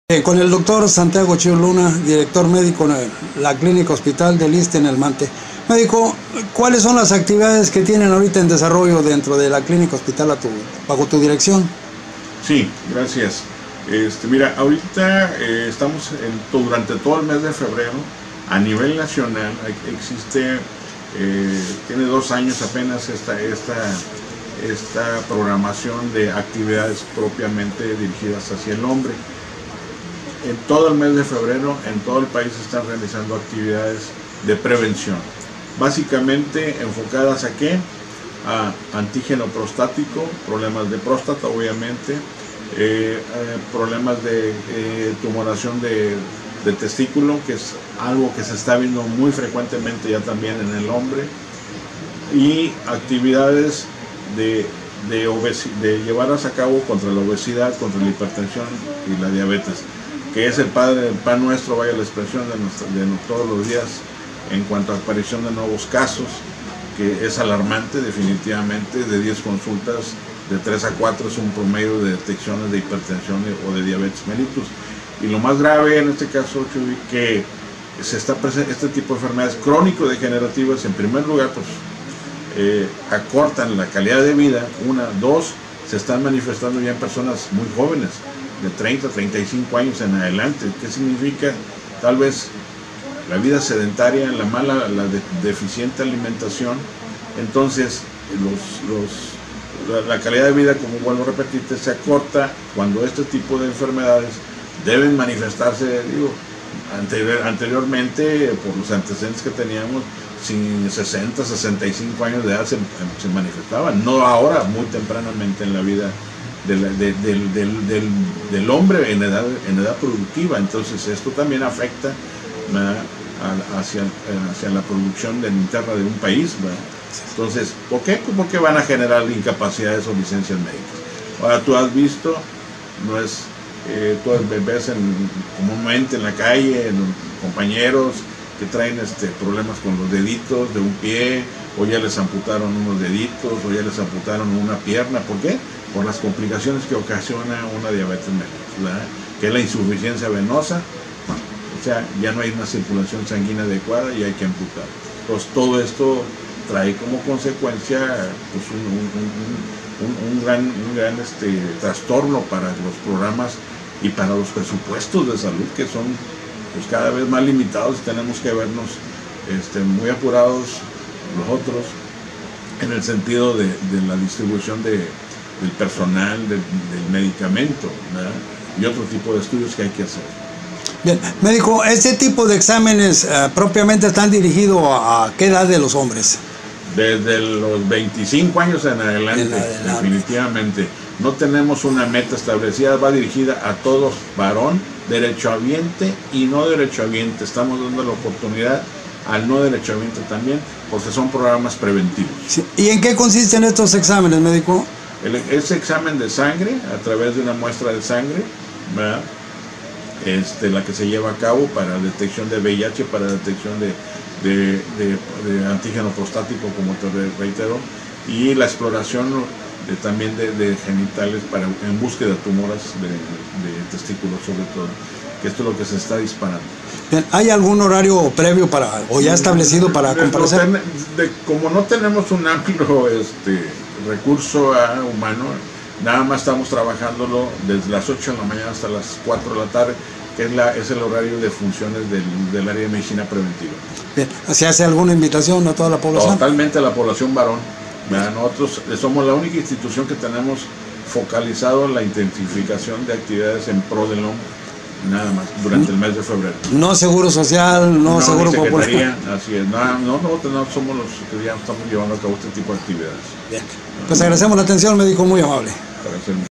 "Hasta los Súper Héroes se Enferman", ISSSTE programa atención...
Febrero el Mes de la Salud del Hombre, un programa de atención  exclusivo del ISSSTE para los varones a nivel nacional, derechohabientes o no, todos serán atendidos gratuitamente...